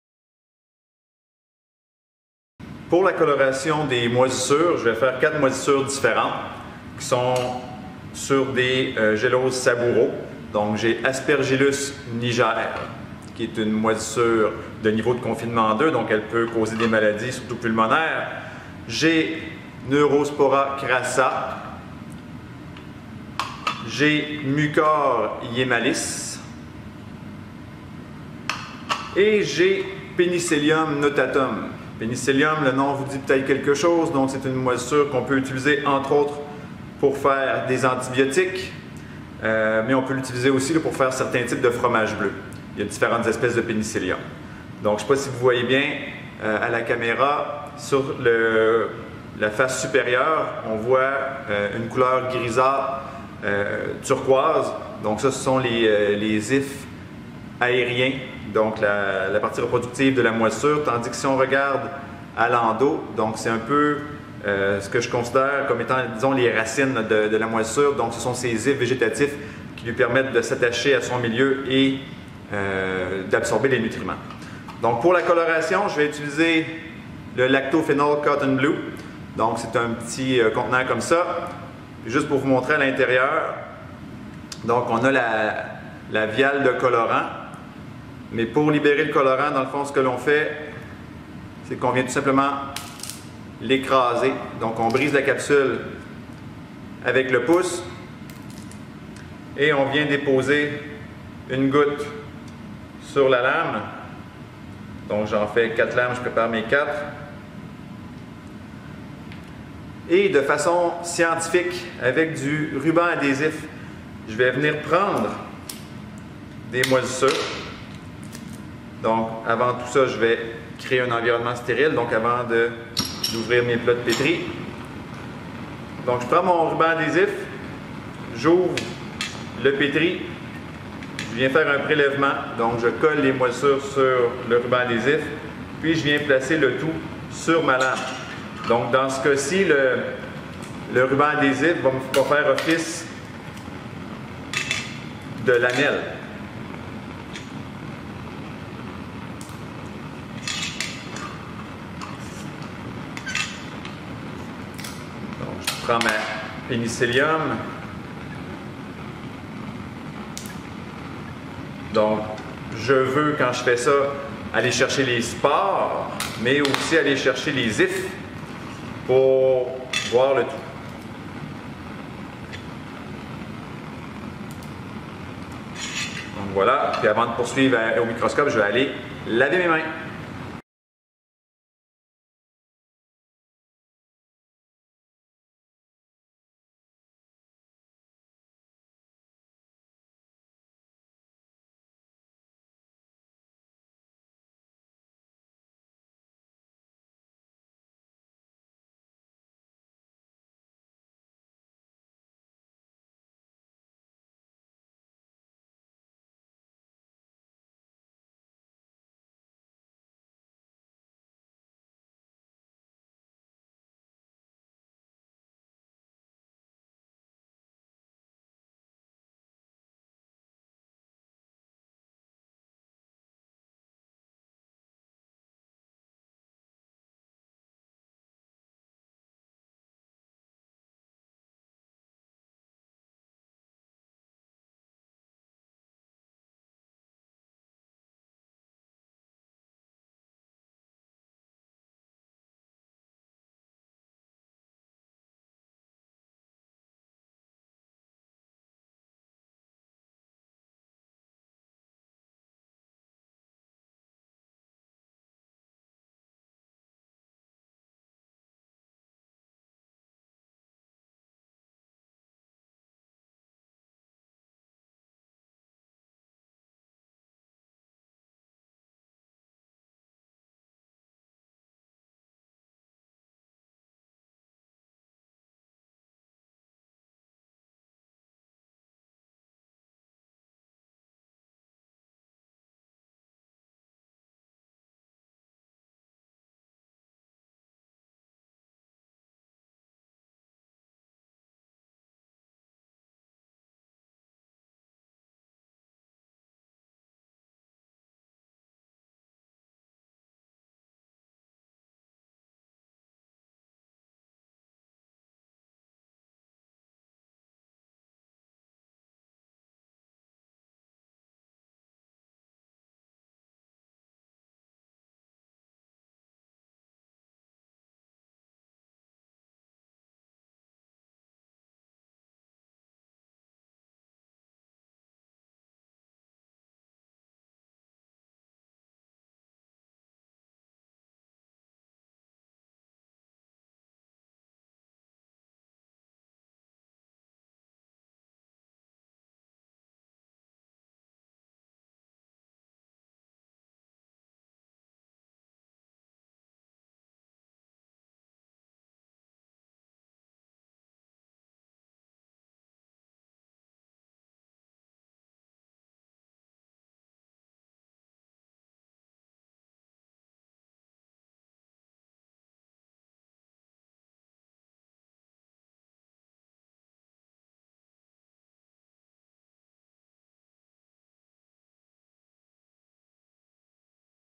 MOISISSURES AU MICROSCOPE (Coloration et observation)
Je prélève des moisissures sur gélose Sabouraud (SDA) puis je les colore avec du lactophenol cotton blue.
Moisissures observées:
1-Aspergillus niger
2-Neurospora crassa
3-Mucor hiemalis
4-Penicillium notatum